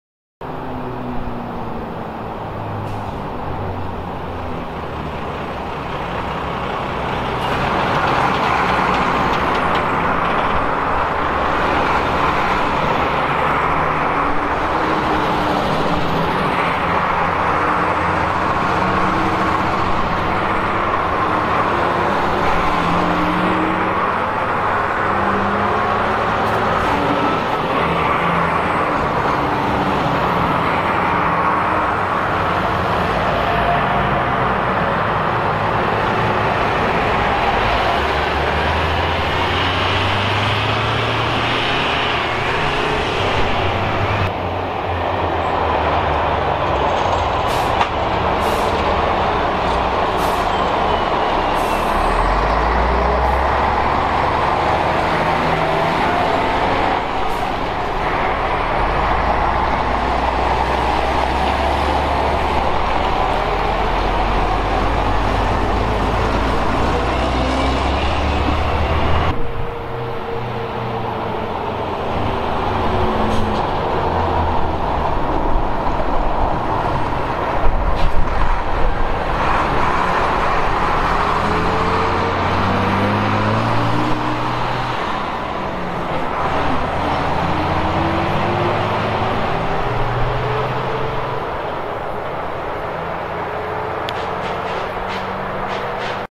Green Griffin 2021- Konvoi Fahrzeuge DSK
Konvoi Fahrzeuge der Division schnelle Kräfte am TrÜbPl Klietz bei der Übung Green Griffin 2021.